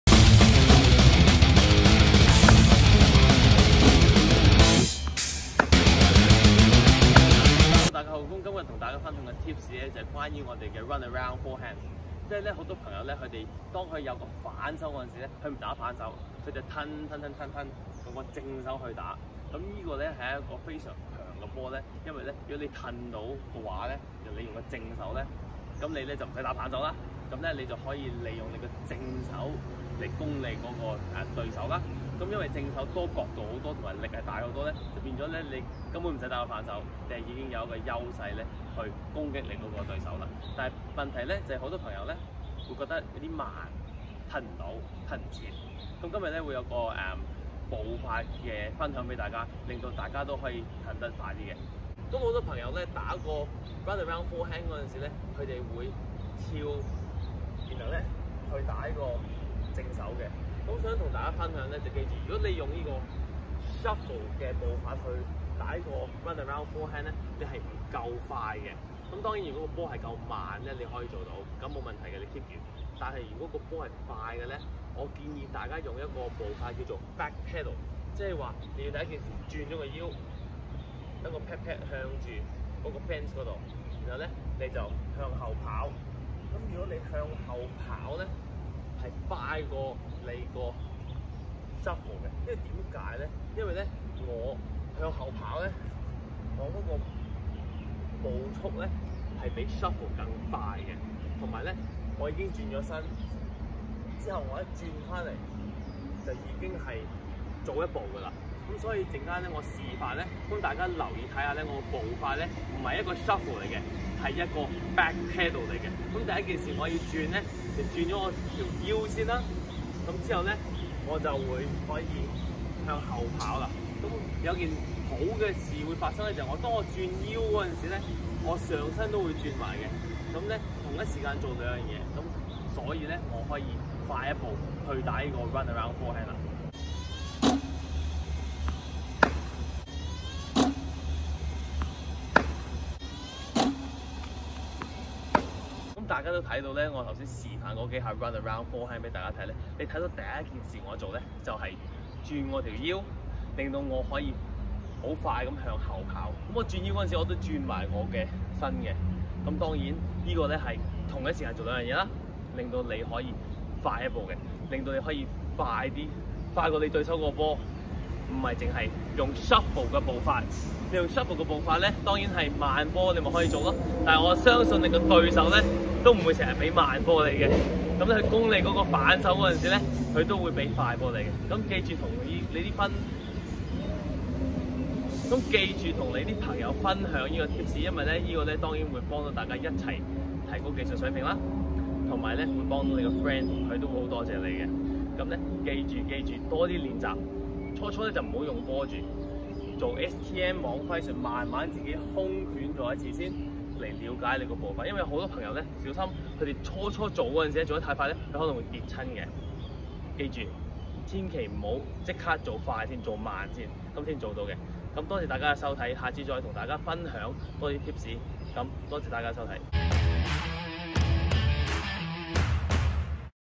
(廣東話) Henry So蘇浩剛國際網球導師及教練 網揮術STM® - Run Around正手步法分享 蘇浩剛國際導師及教練
Henry So蘇浩剛國際網球導師及教練 網揮術STM® - Run Around正手步法分享
蘇浩剛國際導師及教練
請跟分享給你身邊關心的朋友😍😍😍

想要更多簡單而有效的教學資料？